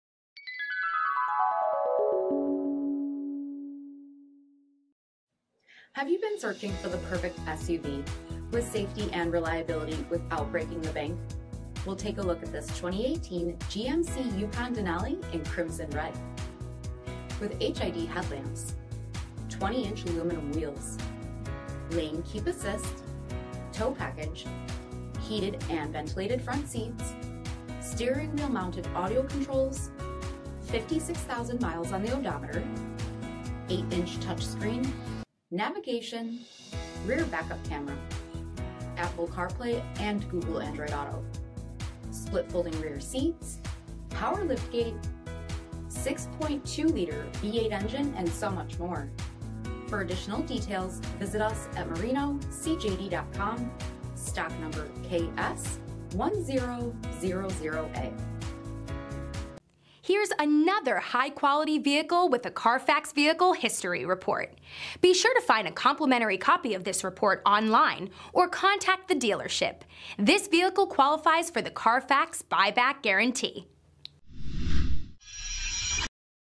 2018 GMC Yukon KS1000A
2018 GMC Yukon Denali

5133 W Irving Park Rd

2018 GMC YUKON DENALI!!  6.2 LITER V8 ENGINE!!   FULL VEHICLE WRAP!!!    8 INCH TOUCH SCREEN DISPLAY!!  NAVIGATION!!  REAR BACKUP CAMERA!!  VENTILATED FRONT SEATS!!  HEATED FRONT SEATS!!  HEATED SECOND ROW SEATS!!  22 INCH ALUMINUM WHEELS!!  POWER LIFTGATE!!  POWER SUNROOF!!  REAR DVD ENTERTAINMENT SYSTEM!!  HEATED STEERING WHEEL!!  REMOTE START SYSTEM!!  TOW PACKAGE!!  ELECTRONIC STABILITY CONTROL!!  POWER LOCKS!!  POWER WINDOWS!!